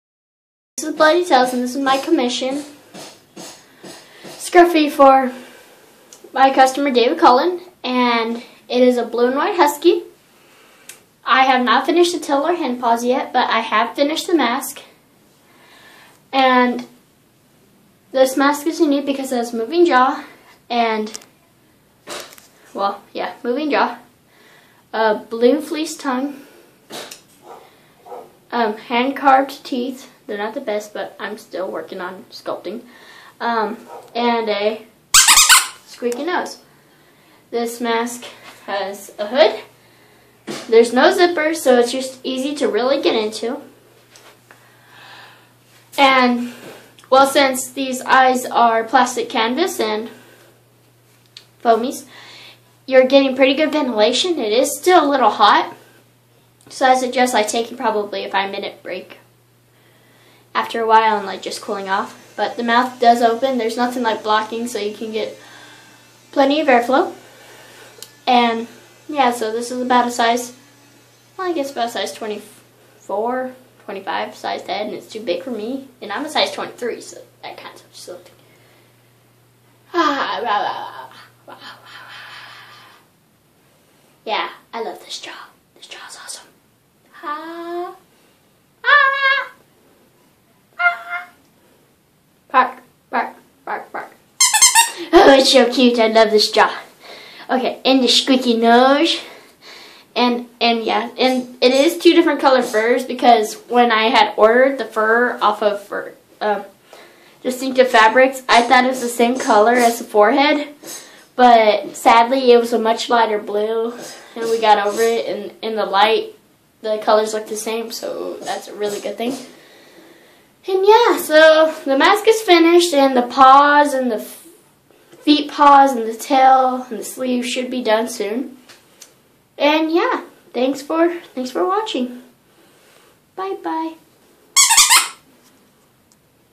Scruffy commission
Not for sale . A blue and white husky commission for David cullen